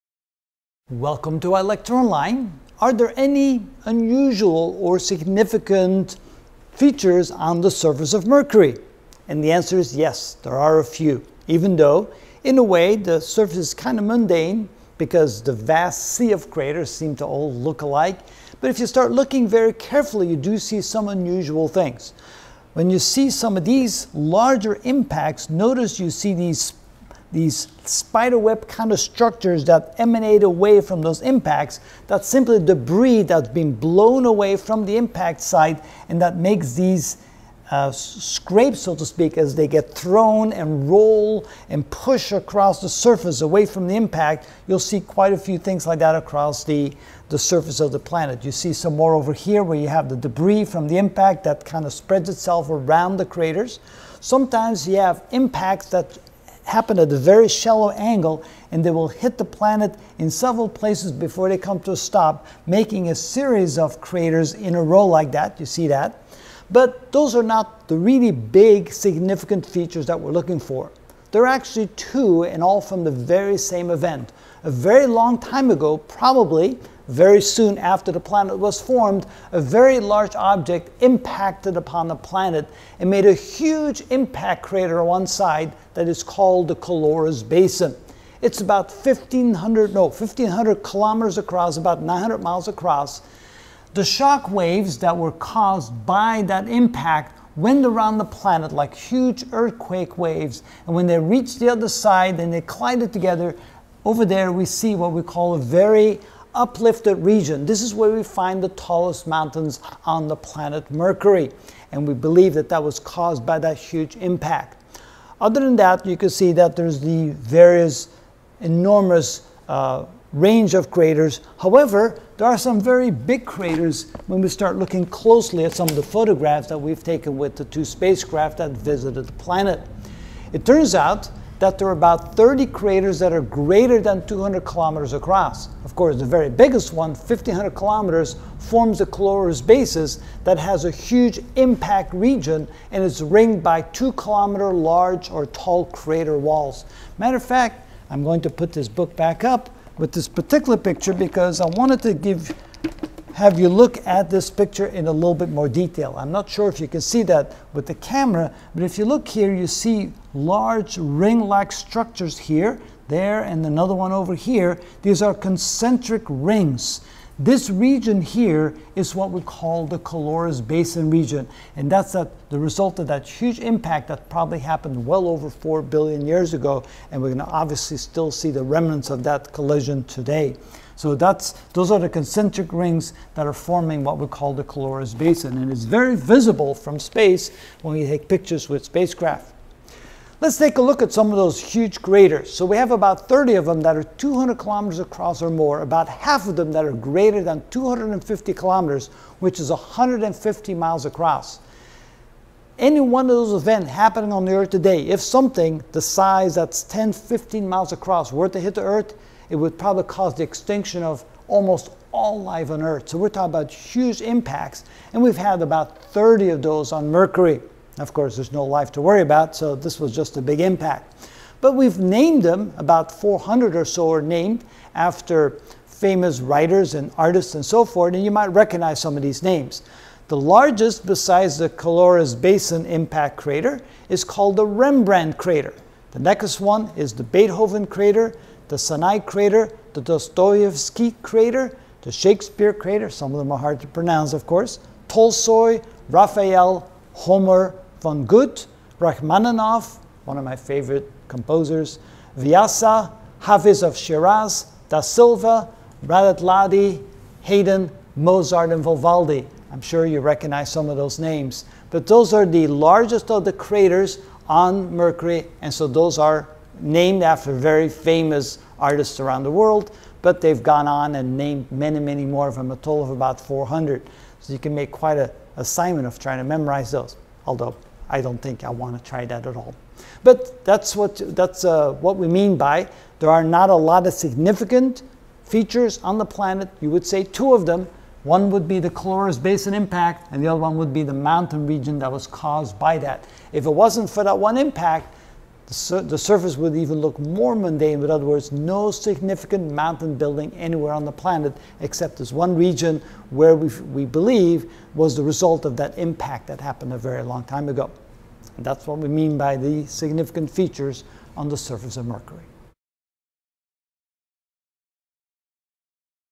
Astronomy - Ch. 10: Mercury (5 of 42) Significant Features
In this video I will discuss the significant features on Mercury. Yes there are “significant features” on Mercury. Even though the surface of may look gray, mundane, and covered with a “sea” of craters. But upon careful examination, unusual features can be observe red.